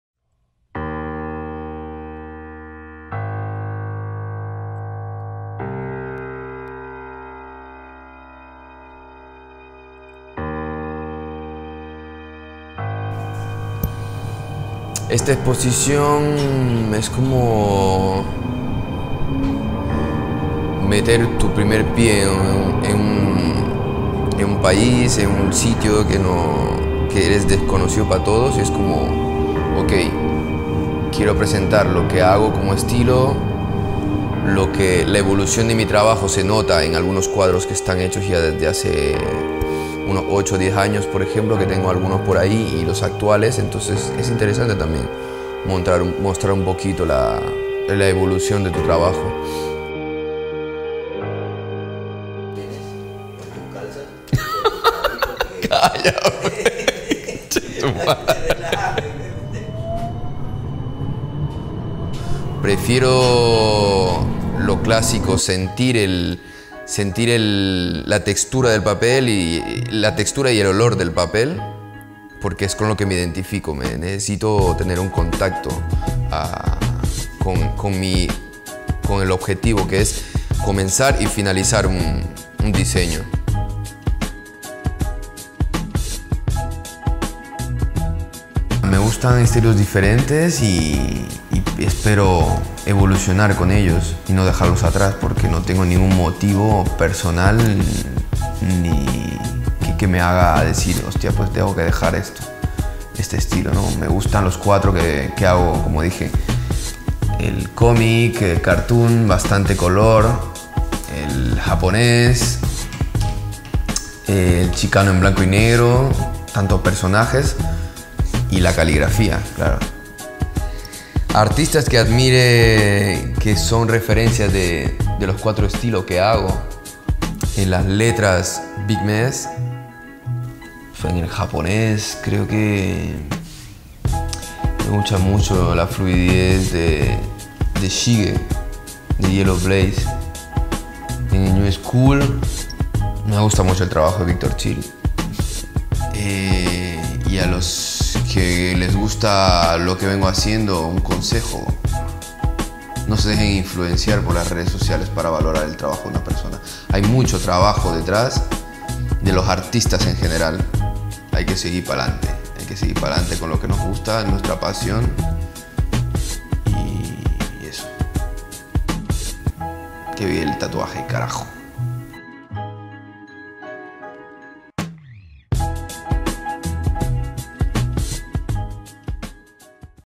« MAKING HEAVY » Expo-Art por Lucho Morante
« Making Heavy » es una Exposición de Arte realizada por Lucho Morante en « Ghostar Ink Gallery » situado en Schaffhausen, Suiza en octubre del 2018.

Agradecer a todos aquellos que han hecho posible tanto el evento como la realización de este visual, un agradecimiento en especial a mi bro Max Castillo (Ghostar) por abrirnos las puertas su su Galería de Arte y Estudio de Tatuajes, por apoyarme y creer en lo que mejor se hacer, mi arte y pasión.

Realización del vídeo: Pedro Toledo
Subtítulos: Blopa

Ghostar Ink Gallery - Switzerland
Blessed Garden Tattoo - France